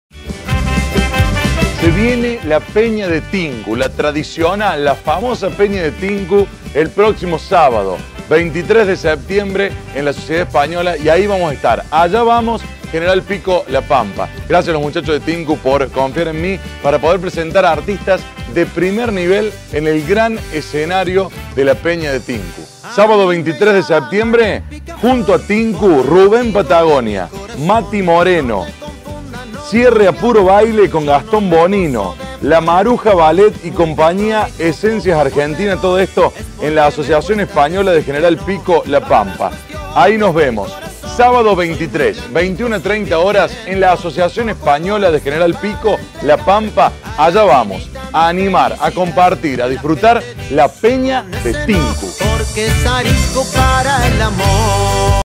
La peña de Tinku - 23 de Septiembre – Sociedad Española - Gral. Pico - La Pampa
23 de Septiembre – Sociedad Española - Gral. Pico - La Pampa
Maestro de Ceremonia: Andrés Bolletta

Realizado por  CincoEFE Productora Audiovisual
Soledad Vega Dini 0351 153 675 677